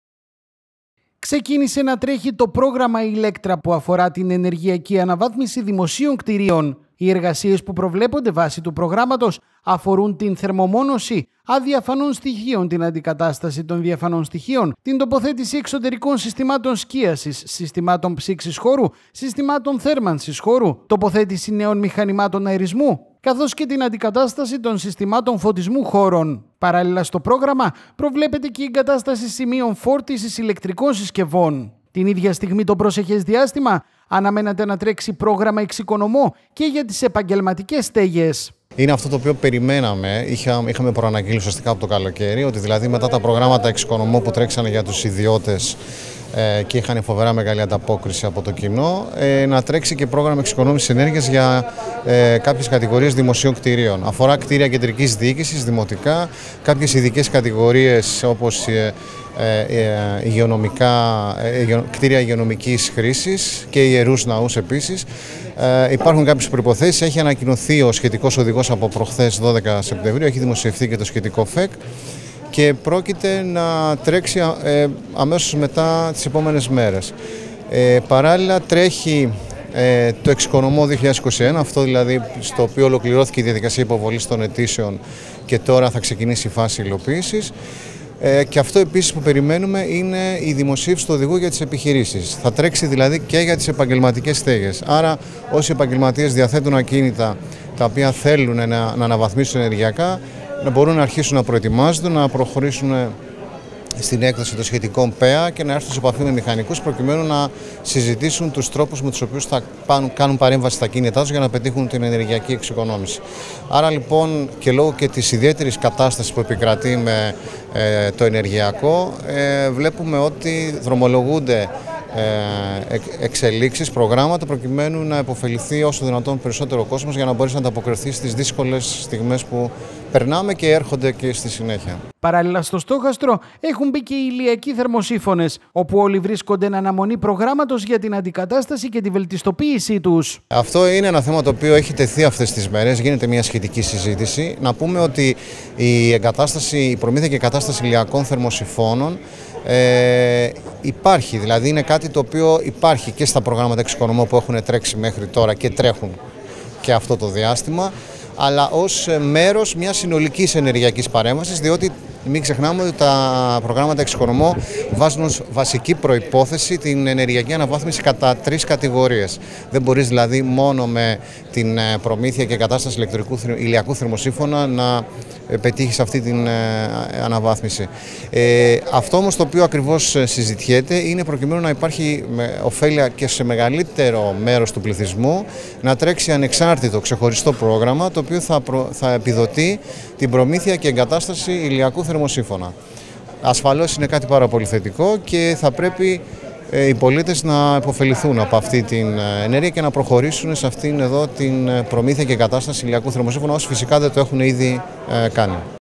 Ξεκίνησε το πρόγραμμα «Ηλέκτρα» για την ενεργειακή αναβάθμιση δημοσίων κτιρίων
Ξεκίνησε το πρόγραμμα «Ηλέκτρα» για την ενεργειακή αναβάθμιση δημοσίων κτιρίων - Έρχεται «εξοικονομώ» για τους ηλιακούς θερμοσίφωνες